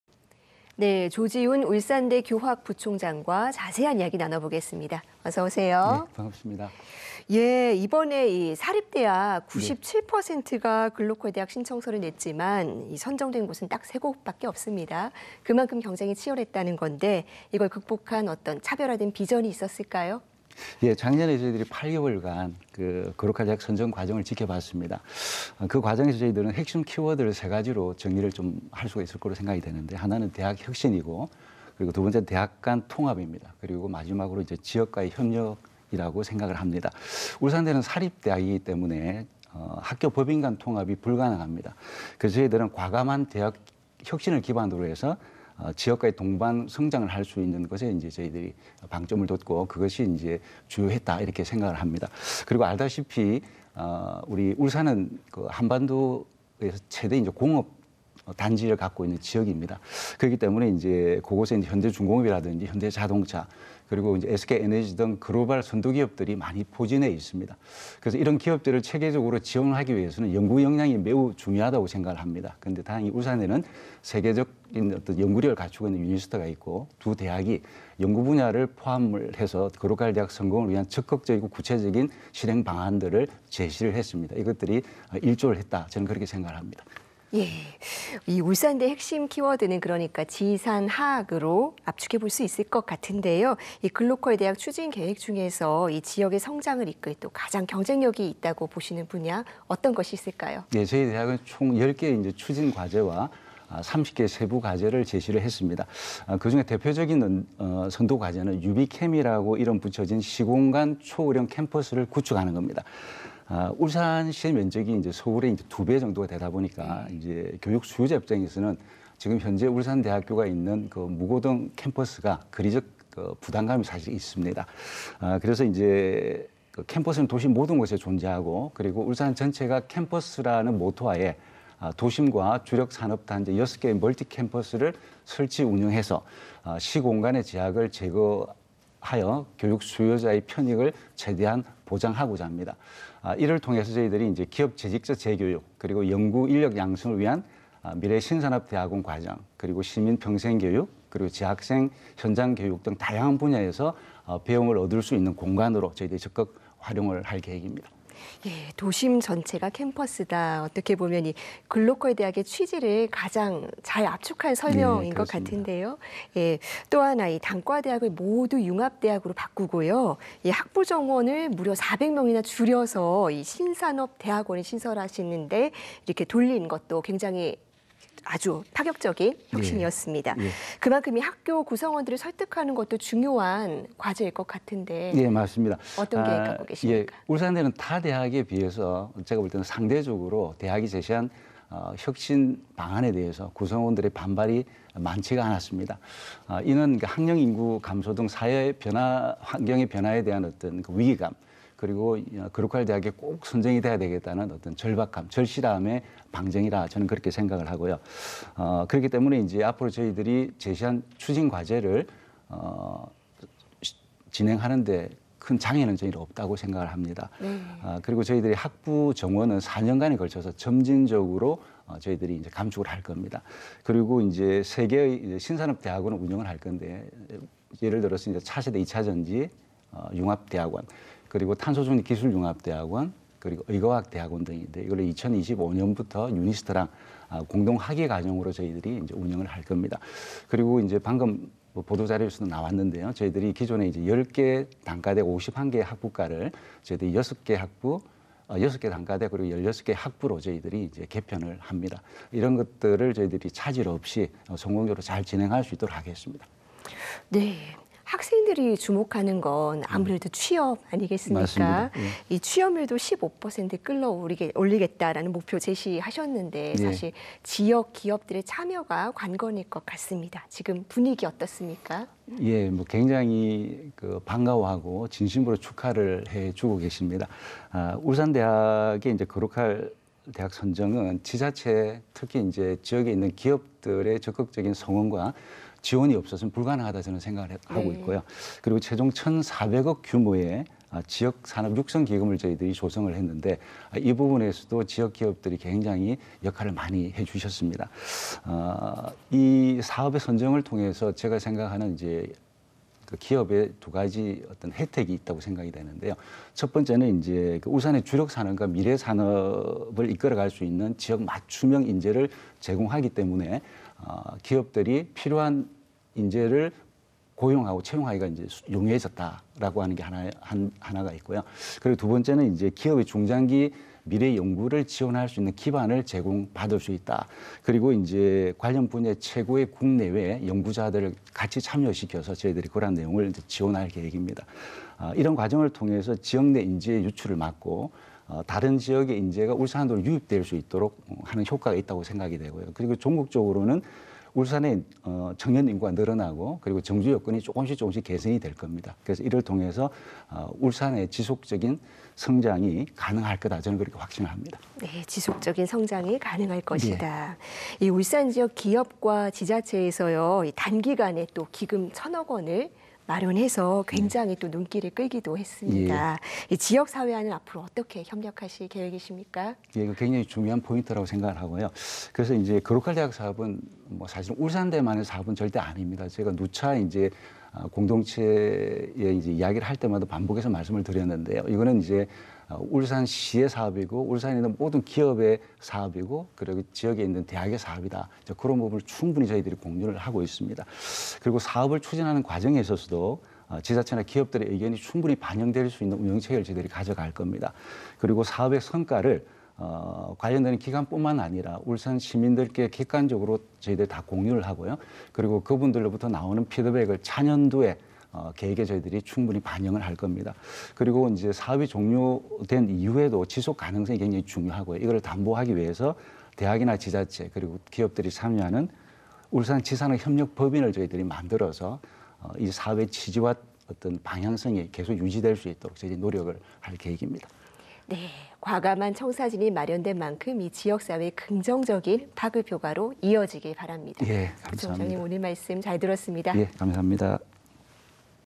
치열한 사립대 경쟁률 뚫은 울산대…글로컬대 선정 비결은? [글로컬대학 기획] / EBS뉴스 2024. 01. 16
[EBS 뉴스]

서현아 앵커
조지운 울산대 교학부총장과 자세한 이야기 나눠보겠습니다.

어서 오세요.

이번에 사립대학 97%가 글로컬대학 신청서를 냈지만 선정된 곳은 딱 세 곳밖에 없습니다.

그만큼 경쟁이 치열했다는 건데 이걸 극복한 어떤 차별화된 비전이 있었을까요?

조지운 교학부총장 / 울산대학교
작년에 저희들이 8개월간 글로컬대학 선정 과정을 지켜봤습니다.

그 과정에서 저희들은 핵심 키워드를 세 가지로 정리를 좀 할 수가 있을 것으로 생각이 되는데 하나는 대학 혁신이고 그리고 두 번째는 대학 간 통합입니다.

그리고 마지막으로 이 지역과의 협력이라고 생각을 합니다.

울산대는 사립대학이기 때문에 학교법인 간 통합이 불가능합니다.

그래서 저희들은 과감한 대학 혁신을 기반으로 해서 지역과의 동반 성장을 할 수 있는 것에 이제 저희들이 방점을 뒀고 그것이 이제 주요했다 이렇게 생각을 합니다.

그리고 알다시피 우리 울산은 한반도에서 최대인 공업단지를 갖고 있는 지역입니다.

그렇기 때문에 이제 그곳에 현대중공업이라든지 현대자동차 그리고 SK에너지 등 글로벌 선도 기업들이 많이 포진해 있습니다.

그래서 이런 기업들을 체계적으로 지원하기 위해서는 연구 역량이 매우 중요하다고 생각을 합니다.

그런데 다행히 울산에는 세계적인 어떤 연구력을 갖추고 있는 UNIST가 있고 두 대학이 연구 분야를 포함을 해서 글로컬대학 성공을 위한 적극적이고 구체적인 실행 방안들을 제시를 했습니다.

이것들이 일조를 했다.

저는 그렇게 생각을 합니다.

서현아 앵커
예 울산대 핵심 키워드는 그러니까 '지산학'으로 압축해 볼 수 있을 것 같은데요.

이 글로컬대학 추진 계획 중에서 이 지역의 성장을 이끌 또 가장 경쟁력이 있다고 보시는 분야, 어떤 것이 있을까요?

네 저희 대학은 총 10개의 추진 과제와 30개의 세부 과제를 제시를 했습니다.

그중에 대표적인 선도 과제는 'UbiCam'이라고 이름 붙여진 시공간 초월형 캠퍼스를 구축하는 겁니다.

울산시의 면적이 서울의 2배 정도가 되다 보니까 이제 교육 수요자 입장에서는 지금 현재 울산대학교가 있는 그 무거동 캠퍼스가 거리적 부담감이 사실 있습니다.

그래서 이제 그 캠퍼스는 도시의 모든 곳에 존재하고 그리고 울산 전체가 캠퍼스라는 모토하에 도심과 주력 산업단지 6개의 멀티 캠퍼스를 설치 운영해서 시공간의 제약을 제거하여 교육 수요자의 편익을 최대한 보장하고자 합니다.

이를 통해서 저희들이 이제 기업 재직자 재교육 그리고 연구 인력 양성을 위한 미래 신산업 대학원 과정 그리고 시민 평생교육 그리고 재학생 현장교육 등 다양한 분야에서 배움을 얻을 수 있는 공간으로 저희들이 적극 활용을 할 계획입니다.

서현아 앵커
도심 전체가 캠퍼스다 어떻게 보면 이 글로컬대학의 취지를 가장 잘 압축한 설명인 것 같은데요.

또 하나 단과대학을 모두 융합대학으로 바꾸고요.

학부 정원을 무려 400명이나 줄여서 신산업 대학원을 신설하시는데 이렇게 돌린 것도 굉장히 아주 파격적인 혁신이었습니다.

그만큼 이 학교 구성원들을 설득하는 것도 중요한 과제일 것 같은데 어떤 계획하고 계신가요?

네 맞습니다.

울산대는 타 대학에 비해서 제가 볼 때는 상대적으로 대학이 제시한 혁신 방안에 대해서 구성원들의 반발이 많지가 않았습니다.

이는 학령인구 감소 등 사회 변화 환경의 변화에 대한 어떤 위기감, 그리고 글로컬대학에 꼭 선정이 돼야 되겠다는 어떤 절박함, 절실함에 방증이라 저는 그렇게 생각을 하고요.

그렇기 때문에 이제 앞으로 저희들이 제시한 추진 과제를 진행하는 데 큰 장애는 저희는 없다고 생각을 합니다.

그리고 저희들이 학부 정원은 4년간에 걸쳐서 점진적으로 저희들이 감축을 할 겁니다.

그리고 이제 3개의 신산업 대학원을 운영을 할 건데 예를 들어서 이제 차세대 이차전지융합대학원 그리고 탄소중립기술융합대학원 그리고 의과학대학원 등인데 이거를 2025년부터 UNIST랑 공동학위 과정으로 저희들이 이제 운영을 할 겁니다.

그리고 이제 방금 보도자료에서도 나왔는데요.

저희들이 기존에 10개 단과대학 51개 학부과를 저희들이 6개 학부 6개 단과대 그리고 16개 학부로 저희들이 개편을 합니다.

이런 것들을 저희들이 차질 없이 성공적으로 잘 진행할 수 있도록 하겠습니다.

서현아 앵커
학생들이 주목하는 건 아무래도 취업 아니겠습니까?

취업률도 15% 끌어 올리겠다는 목표 제시하셨는데 사실 지역 기업들의 참여가 관건일 것 같습니다.

지금 분위기 어떻습니까?

예 굉장히 반가워하고 진심으로 축하를 해 주고 계십니다.

울산대학의 글로컬대학 선정은 지자체 특히 지역에 있는 기업들의 적극적인 성원과 지원이 없어서는 불가능하다 저는 생각을 하고 있고요.

그리고 최종 1,400억 규모의 지역 산업 육성기금을 저희들이 조성을 했는데 이 부분에서도 지역 기업들이 굉장히 역할을 많이 해 주셨습니다.

이 사업의 선정을 통해서 제가 생각하는 이제 기업의 두 가지 어떤 혜택이 있다고 생각이 되는데요.

첫 번째는 이제 울산의 주력산업과 미래산업을 이끌어 갈 수 있는 지역 맞춤형 인재를 제공하기 때문에 기업들이 필요한 인재를 고용하고 채용하기가 용이해졌다라고 하는 게 하나가 있고요.

그리고 두 번째는 이제 기업의 중장기 미래 연구를 지원할 수 있는 기반을 제공받을 수 있다.

그리고 이제 관련 분야 최고의 국내외 연구자들을 같이 참여시켜서 저희들이 그러한 내용을 지원할 계획입니다.

이런 과정을 통해서 지역 내 인재 유출을 막고 다른 지역의 인재가 울산으로 유입될 수 있도록 하는 효과가 있다고 생각이 되고요.

그리고 전극적으로는 울산의 청년 인구가 늘어나고 그리고 정주 여건이 조금씩 조금씩 개선이 될 겁니다.

그래서 이를 통해서 울산의 지속적인 성장이 가능할 거다.

저는 그렇게 확신을 합니다.

서현아 앵커
지속적인 성장이 가능할 것이다.

울산지역 기업과 지자체에서 단기간에 또 기금 1천억 원을 마련해서 굉장히 또 눈길을 끌기도 했습니다.

지역 사회와는 앞으로 어떻게 협력하실 계획이십니까?

굉장히 중요한 포인트라고 생각을 하고요.

그래서 이제 글로컬대학 사업은 사실은 울산대만의 사업은 절대 아닙니다.

저희가 누차 이제 공동체의 이야기를 할 때마다 반복해서 말씀을 드렸는데요.

이거는 이제 울산시의 사업이고 울산에 있는 모든 기업의 사업이고 그리고 지역에 있는 대학의 사업이다 그런 부분을 충분히 저희들이 공유를 하고 있습니다.

그리고 사업을 추진하는 과정에 있어서도 지자체나 기업들의 의견이 충분히 반영될 수 있는 운영 체계를 저희들이 가져갈 겁니다.

그리고 사업의 성과를 관련된 기관뿐만 아니라 울산시민들께 객관적으로 저희들이 다 공유를 하고요.

그리고 그분들로부터 나오는 피드백을 차년도에 계획에 저희들이 충분히 반영을 할 겁니다.

그리고 이제 사업이 종료된 이후에도 지속 가능성이 굉장히 중요하고요.

이걸 담보하기 위해서 대학이나 지자체 그리고 기업들이 참여하는 '울산지산학협력법인'을 저희들이 만들어서 이 사업의 취지와 어떤 방향성이 계속 유지될 수 있도록 저희들이 노력을 할 계획입니다.

서현아 앵커
과감한 청사진이 마련된 만큼 지역 사회의 긍정적인 파급 효과로 이어지길 바랍니다.

총장님 오늘 말씀 잘 들었습니다.